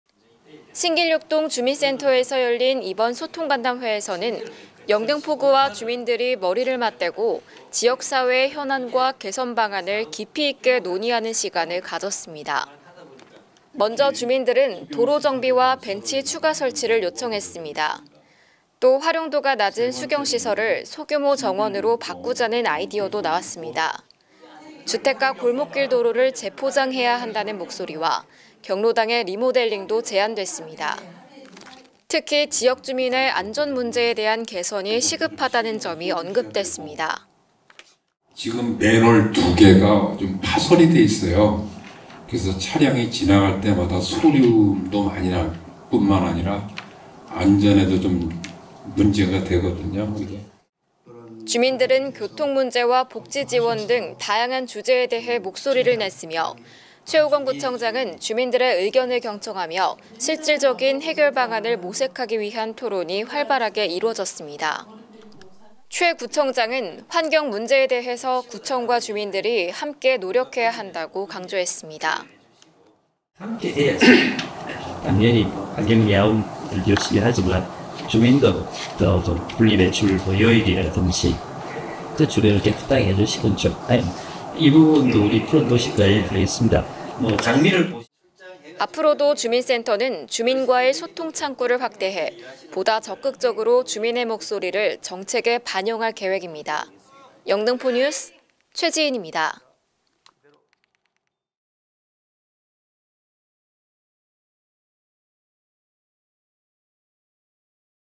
찾아가는 동 소통간담회 _ 신길6동
신길6동 주민센터에서 열린 이번 소통간담회에서는
영등포구와 주민들이 머리를 맞대고 지역 사회의 현안과
개선 방안을 깊이 있게 논의하는 시간을 가졌습니다.
[신길6동, 동 소통간담회 진행]

먼저 주민들은 도로 정비와 벤치 추가 설치를 요청했습니다.
또, 활용도가 낮은 수경 시설을 소규모 정원으로 바꾸자는 아이디어도 나왔습니다.
주택가 골목길 도로를 재포장해야 한다는 목소리와
경로당의 리모델링도 제안됐습니다.
[도로 정비, 골목길 도로 재포장 등 안건 제시]

특히 지역 주민의 안전 문제에 대한 개선이 시급하다는 점이 언급됐습니다.
주민들은 교통 문제와 복지 지원 등 다양한 주제에 대해 목소리를 냈으며,
최호권 구청장은 주민들의 의견을 경청하며
실질적인 해결 방안을 모색하기 위한 토론이 활발히 이뤄졌습니다.
[교통문제, 복지지원 등 다양한 주제로 토론]

최 구청장은 환경 문제에 대해서 구청과 주민들이
함께 노력해야 한다고 강조했습니다.

앞으로도 주민센터는 주민과의 소통 창구를 확대해
보다 적극적으로 주민의 목소리를 정책에 반영할 계획입니다.